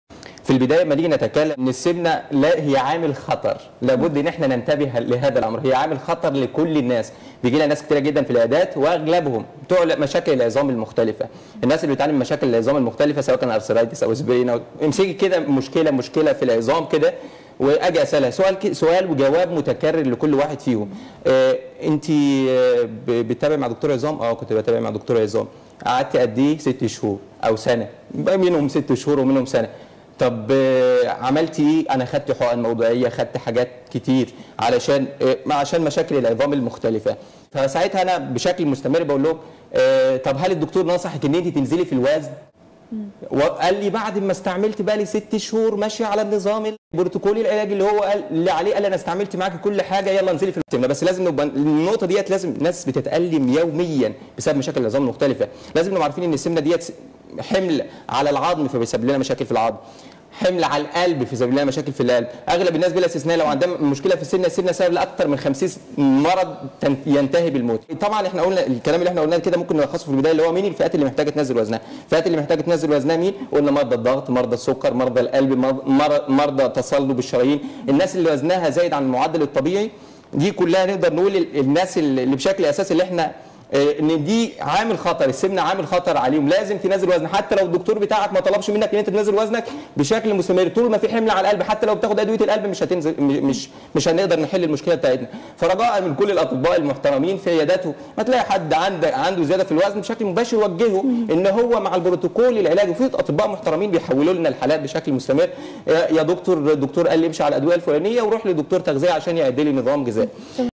خطر|| تنبيه هام$يجب أن تذهب لطبيب تغذية لإنقاص الوزن إذا كنت مريض عظام او قلب او ضغط سكر كوليسترول
شرح مختصر اهمية انقاص الوزن لهذه الحالات المرضيه مع د.احمد عطيه الدهشان